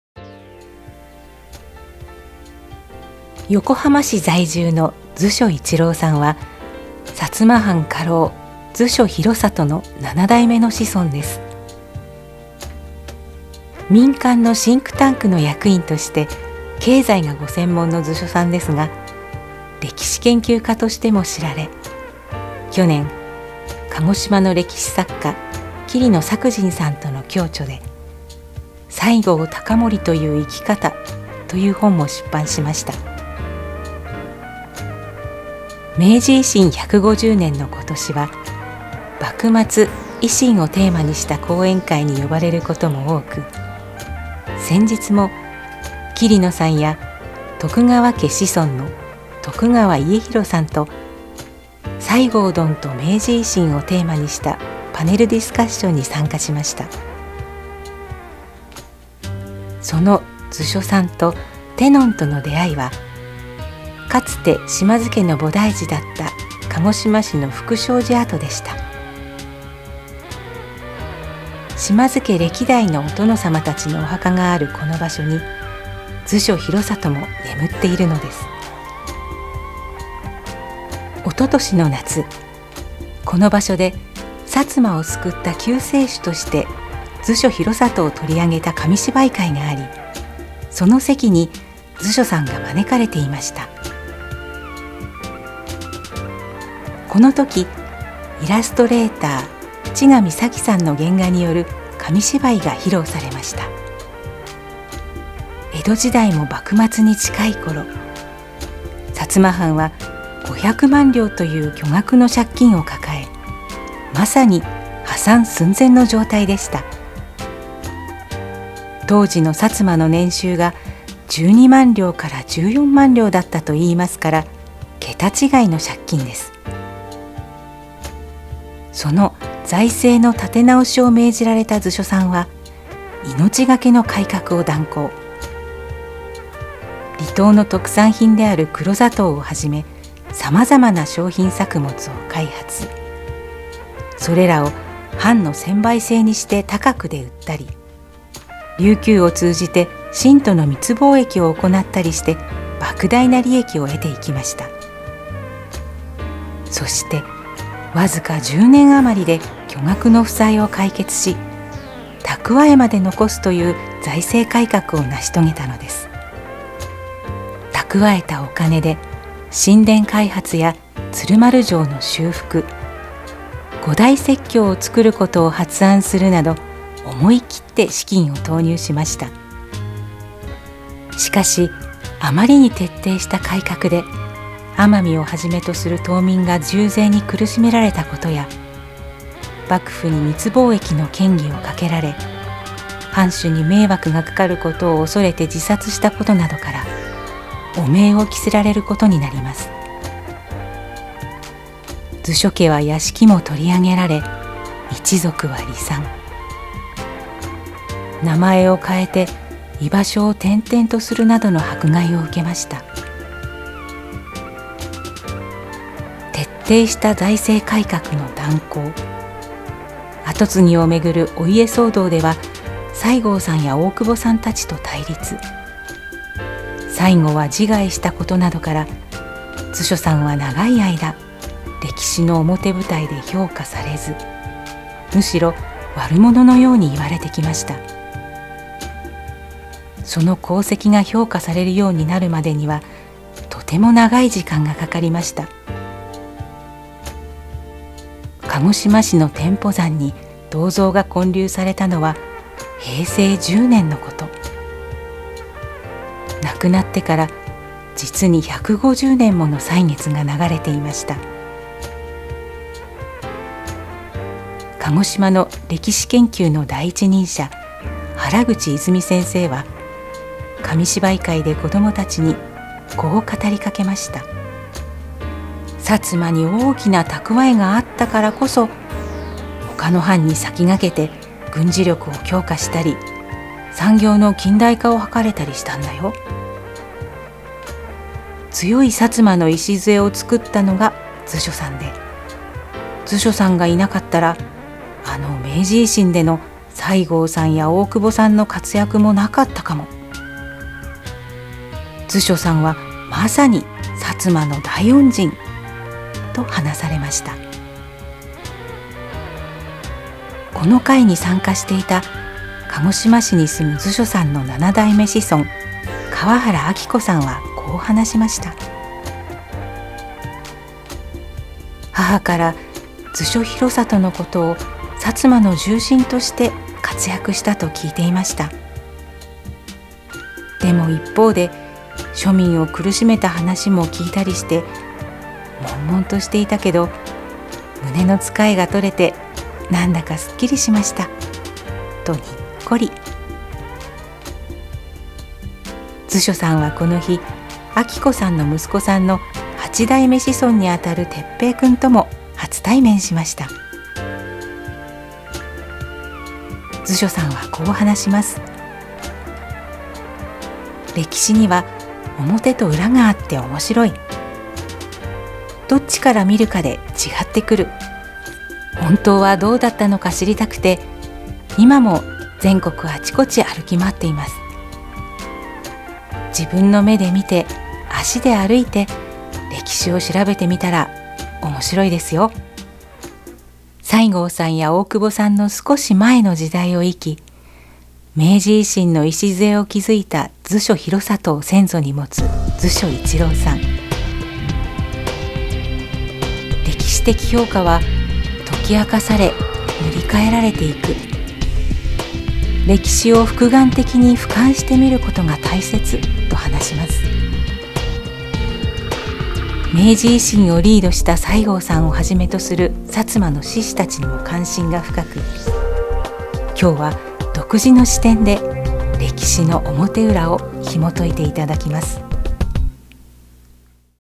調所一郎紹介
「てのん学校」第一回のテーマは、「西郷さんはどうしてすごいのか！」歴史のエキスパートのおふたりをゲストにお迎えすることが出来ました。

薩摩藩家老の調所広郷の7代目子孫・調所一郎さん。調所さんは横浜から駆けつけて下さいました。そして、鹿児島の歴史博物館・尚古集成館の学芸員、小平田史穂さん。

いずれも「てのん」の記事でご紹介させて頂いたおふたり。そのおふたりを紹介するビデオからトークショーが始まりました。